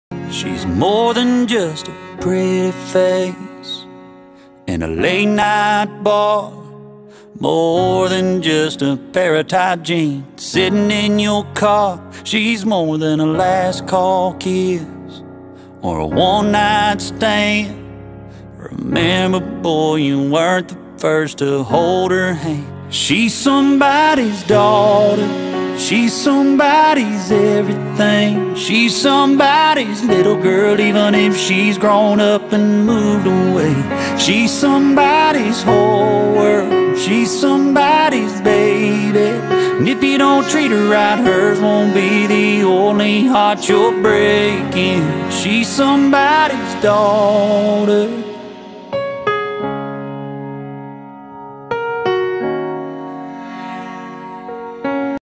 Blasting around on the Carolina Skiff J14
Sometimes it's too beautiful for words out there on Lake Hartwell